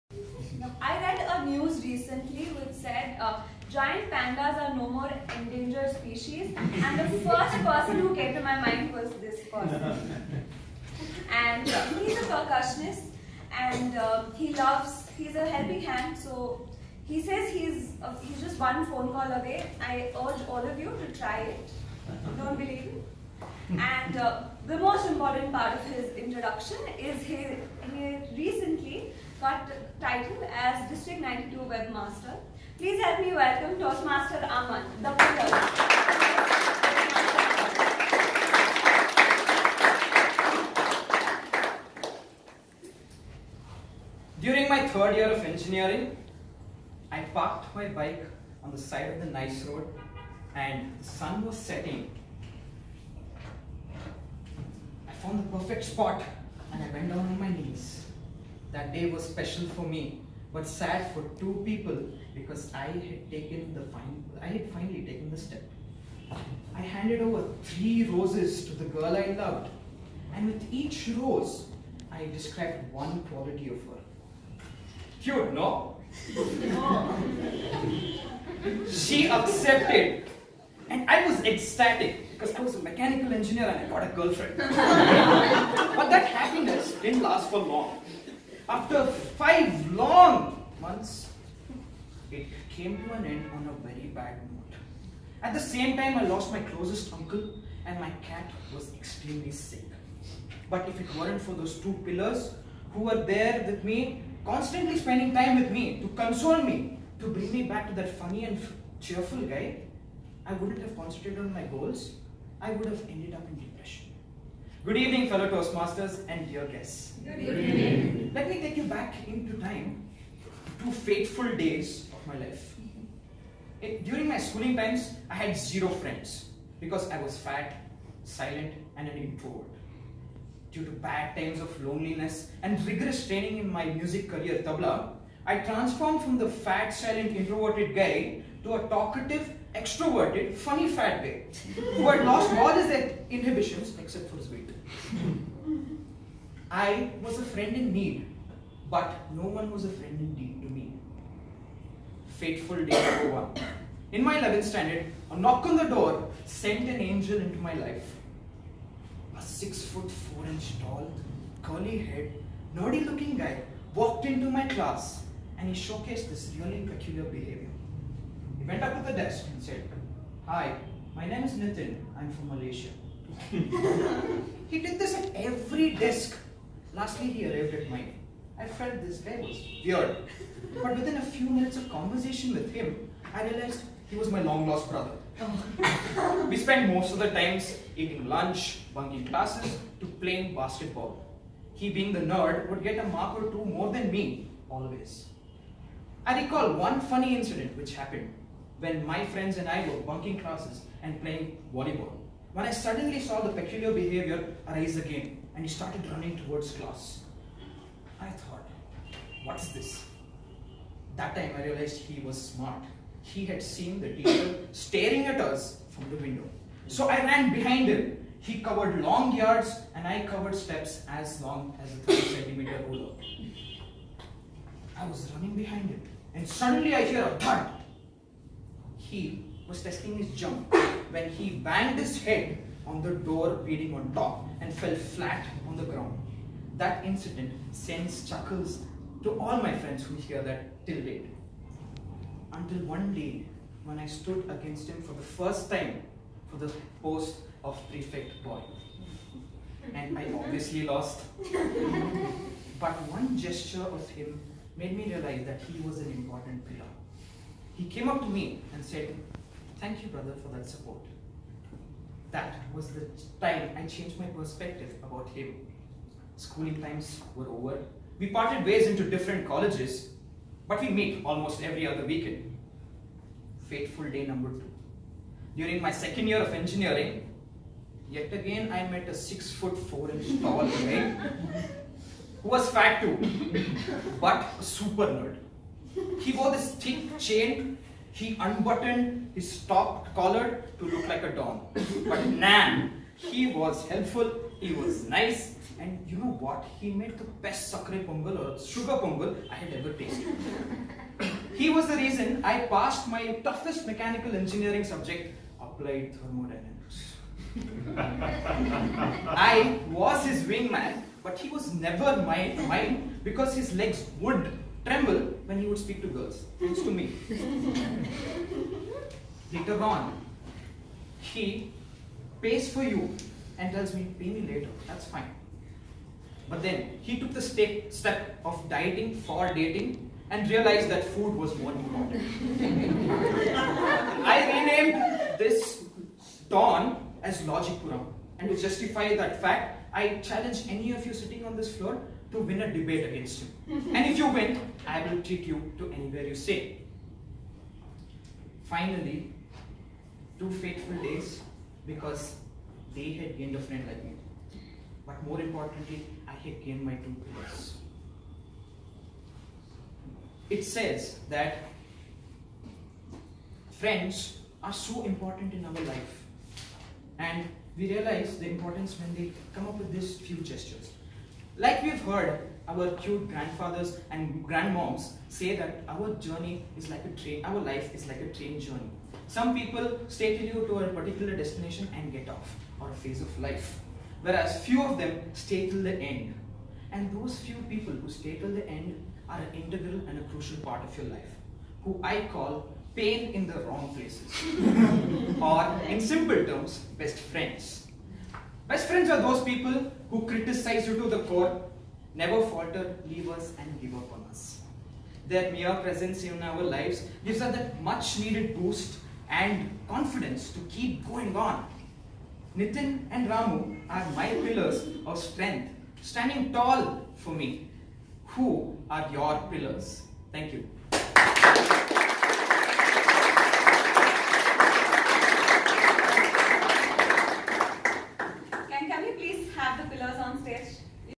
Aman Tayyab- CC5 (Two Pillars)
Body Language: Find out what the pillars mean and how they played an important role in TM Aman's life. He questions you as to who are your pillars and say a Thank you to them...